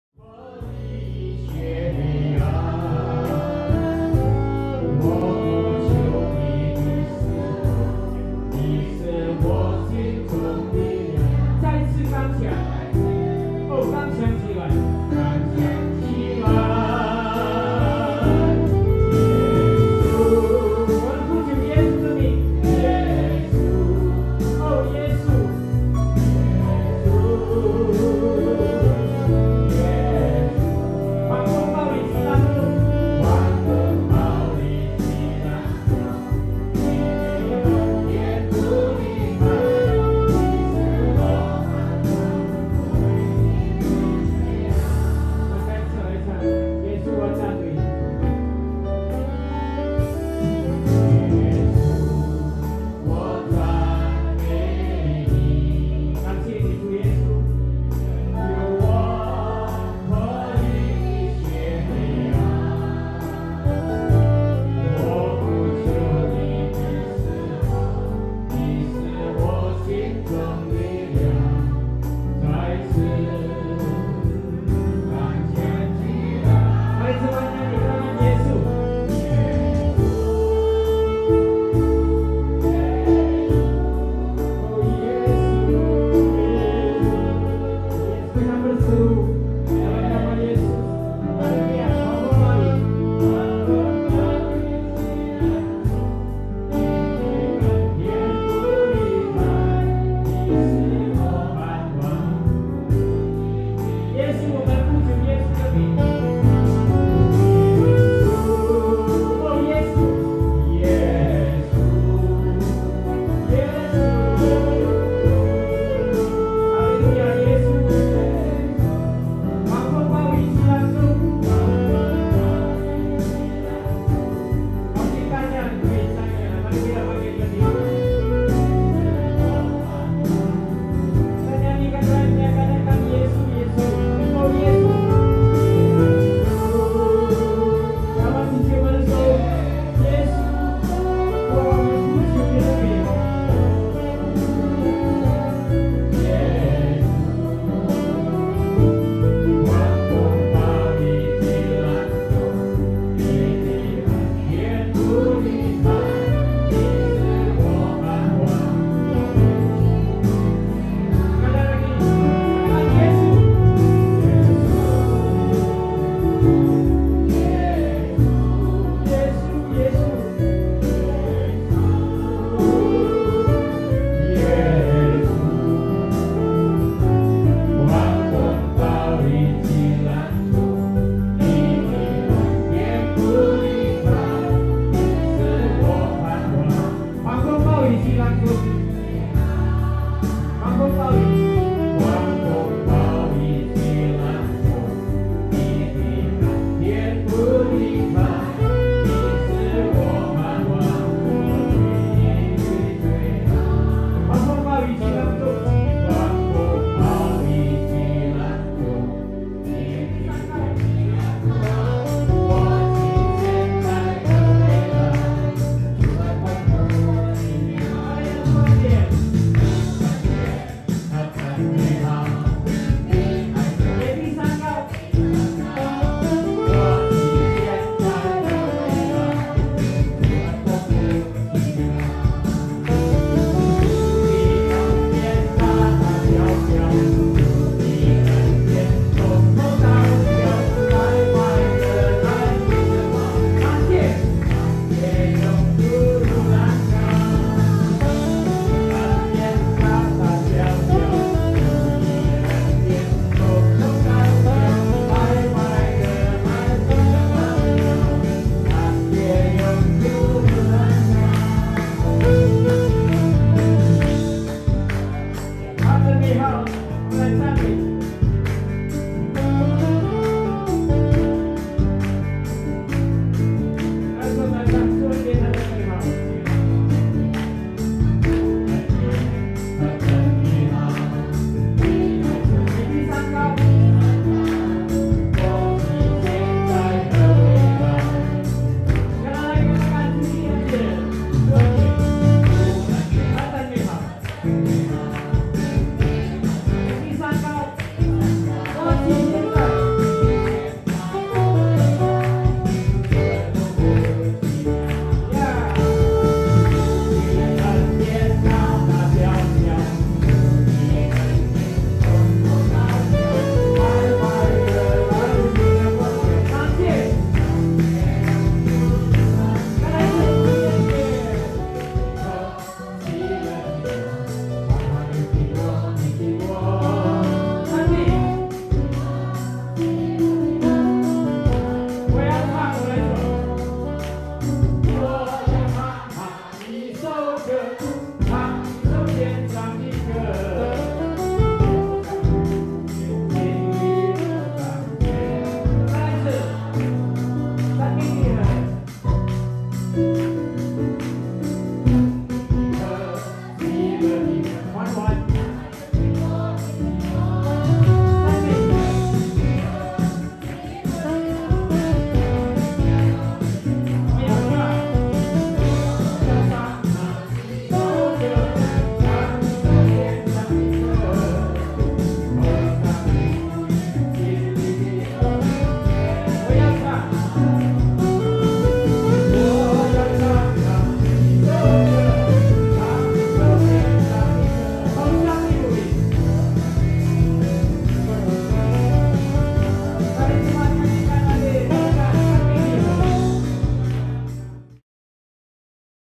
Mandarin Fellowship Service (part 3) - Saxophone - Fave Hotel Puri Indah - May 2019
Snaps from a Mandarin service at Fave Hotel Puri Indah on May 18th, 2019.